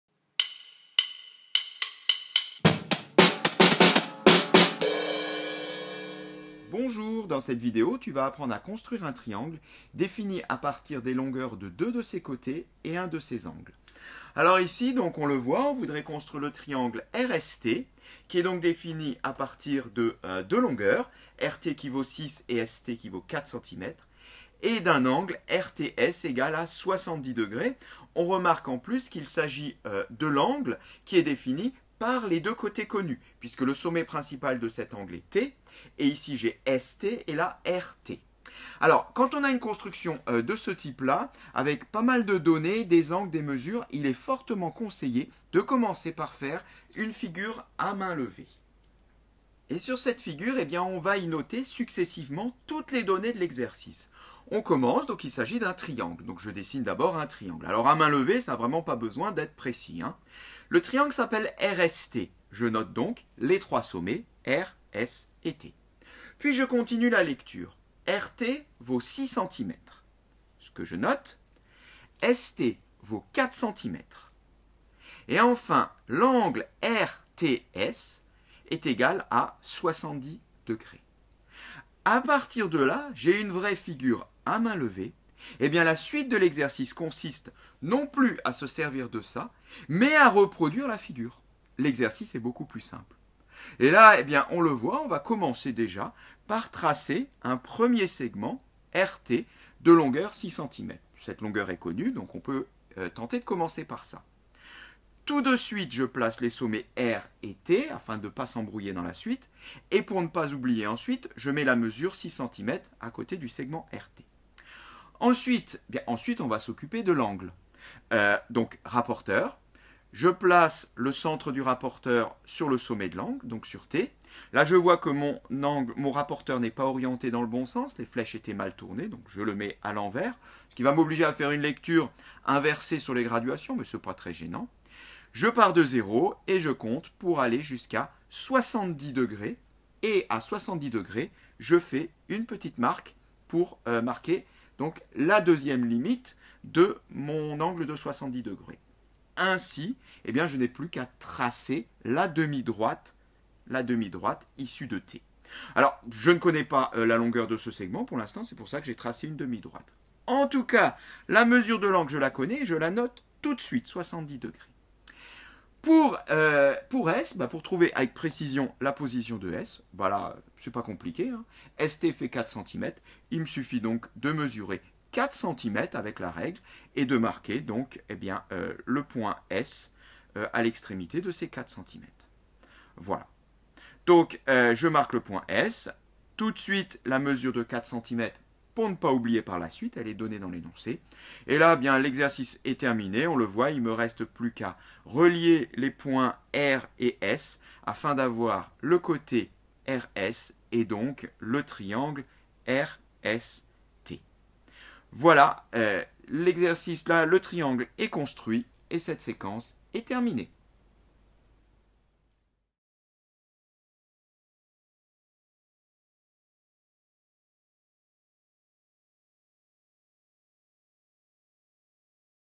Construire un triangle (2) - Cinquième
Dans cette vidéo, tu pourras apprendre à construire un triangle défini par les longueurs de 2 côtés et la mesure d'un angle. 👍